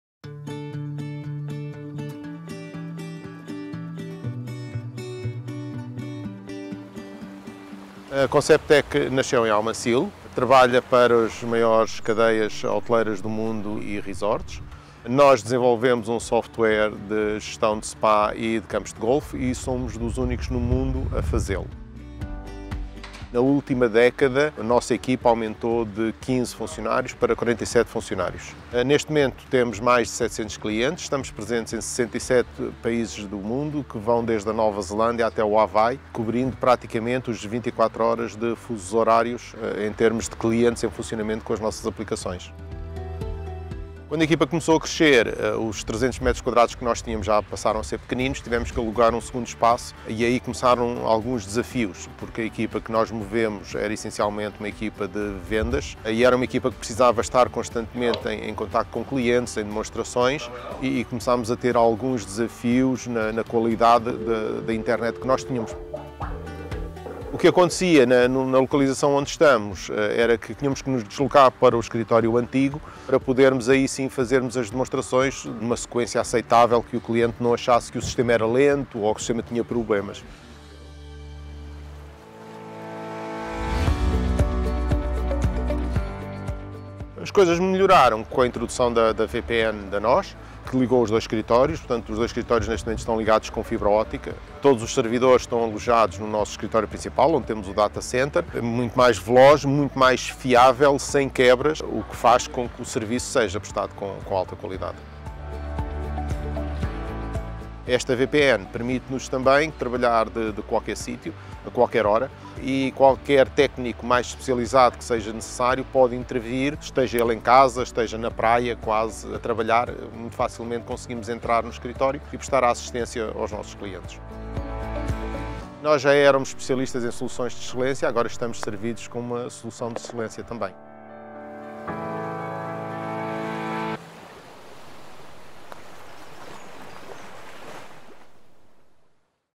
Conceptek | Com a VPN da NOS Empresas, o escritório nunca fecha
A Conceptek é especialista em soluções de gestão de campos de golfe e spa. Na última década, passou de 3 para 47 colaboradores. Quando abriu um segundo escritório, resolveu o problema da falta de espaço, mas criou outro – com um acesso comum, a ligação à rede da empresa era lenta, pouco fiável e pouco segura. Agora, com a VPN da NOS, os dois escritórios funcionam como um só e contam com uma ligação veloz, segura e robusta, que assegura maior produtividade e uma gestão mais eficiente dos projetos dos seus clientes.